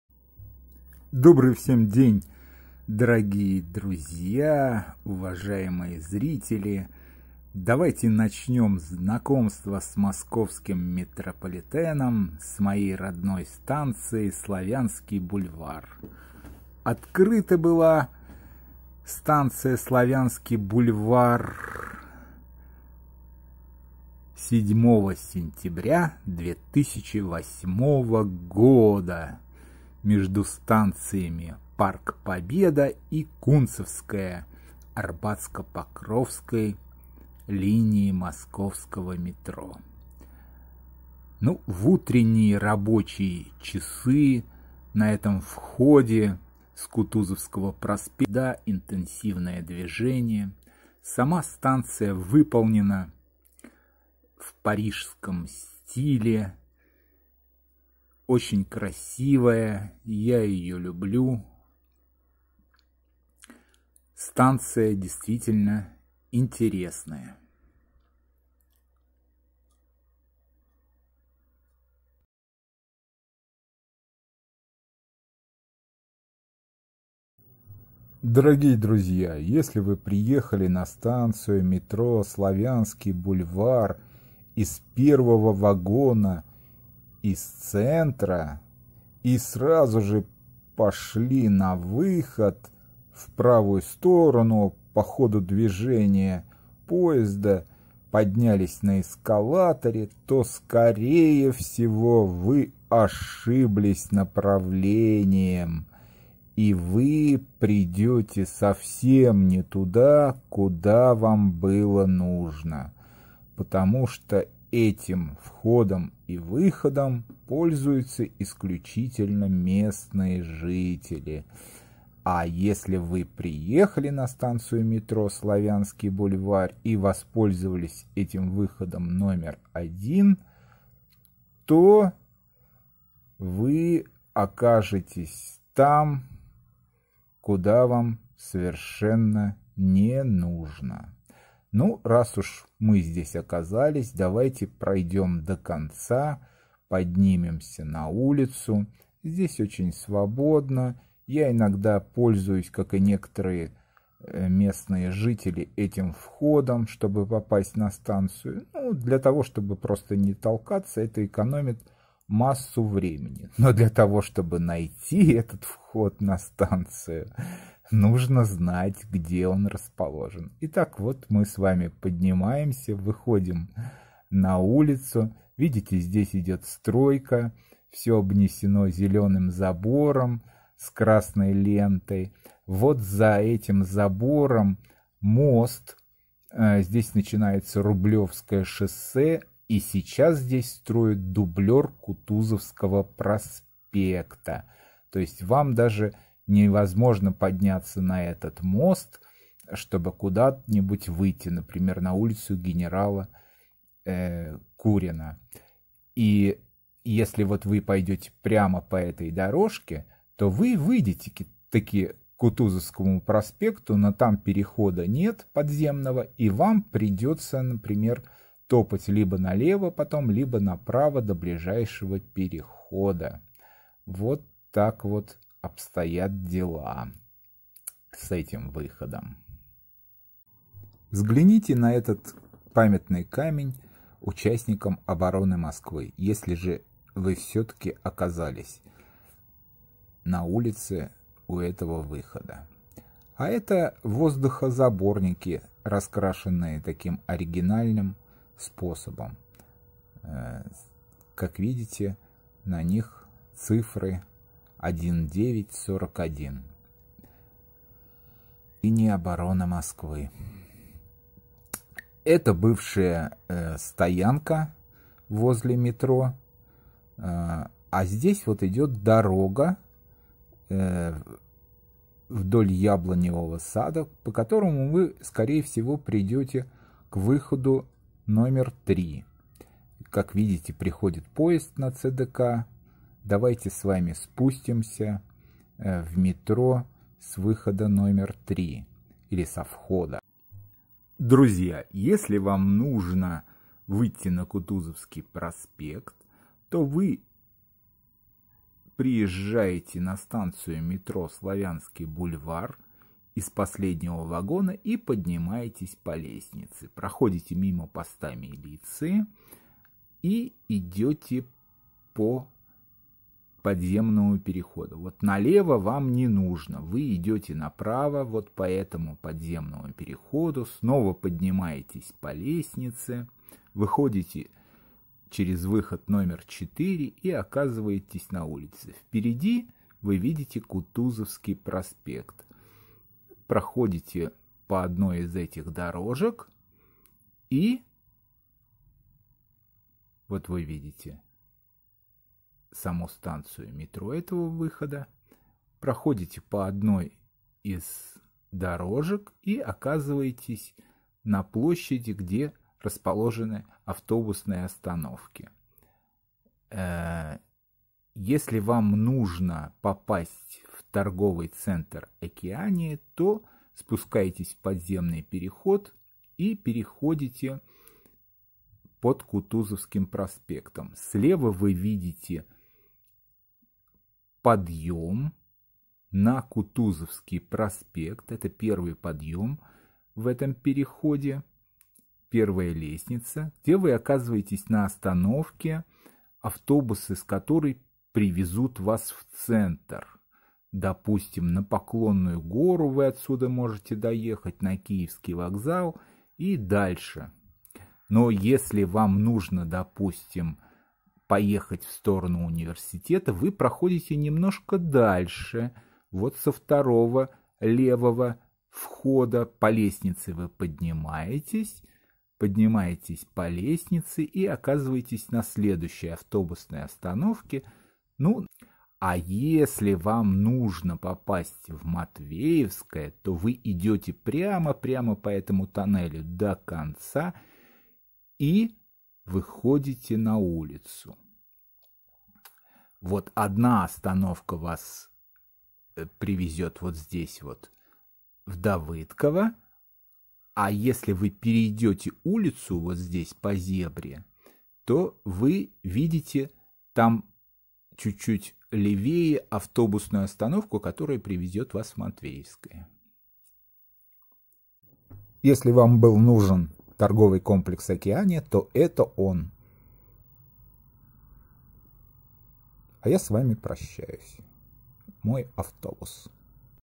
Slavyansky Bulvar metro station
Slavyansky Bulvar metro station. Arbatsko-Pokrovskaya line

00:00       Native station

01:18       If you go to the right, you won't find anything

03:48       Moscow Defense Line

04:50       You will go to the left = you will lose your horse

05:55       If you go straight, you'll go to Oceania